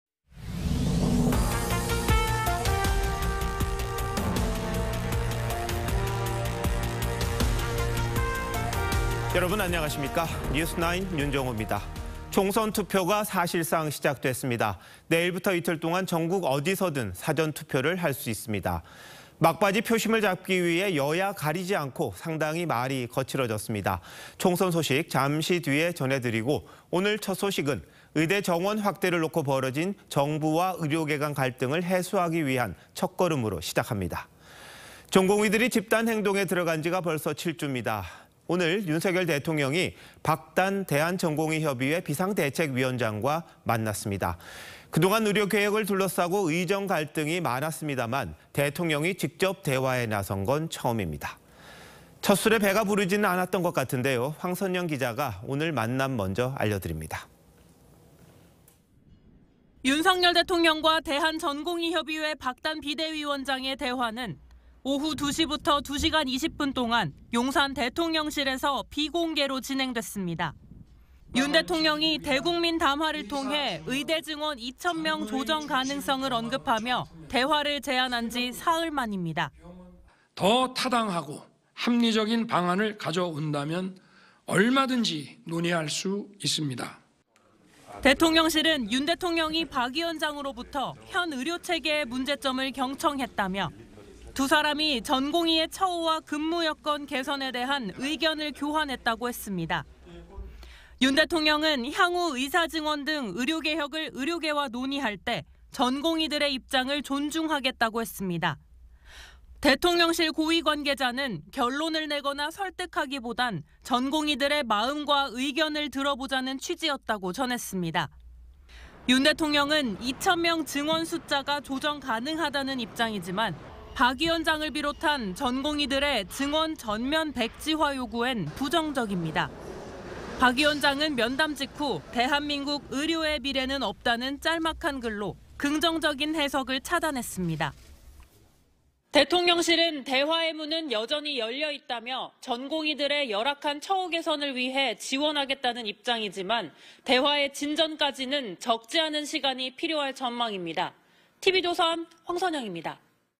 尹, 전공의 대표와 2시간20분 만났지만 접점 못찾아…"의대증원에 전공의 입장 존중"
총선 투표가 사실상 시작됐습니다. 내일부터 이틀 동안 전국 어디서든 사전투표를 할 수 있습니다. 막바지 표심을 잡기 위해 여야 가리지 않고 상당히 거칠어졌는데, 총선 소식은 잠시 뒤에 전해드리고, 오늘 첫 소식은 의대 정원 확대를 놓고 벌어진 정부와 의료계간 갈등을 해소하기 위한 첫 걸음으로 시작합니다.

전공의 들이 집단행동에 들어간 지 7주 만에 윤석열 대통령이, 박단 대한전공의협의회 비상대책위원장과 만났습니다. 그동안 의료개혁을 둘러싸고 의정 갈등이 많았지만, 대통령이 직접 대화에 나선 건 처음입니다.

첫 술에 배가 부르진 못했던 것 같은데, 황선영 기자가 오늘 만남부터 알려드립니다.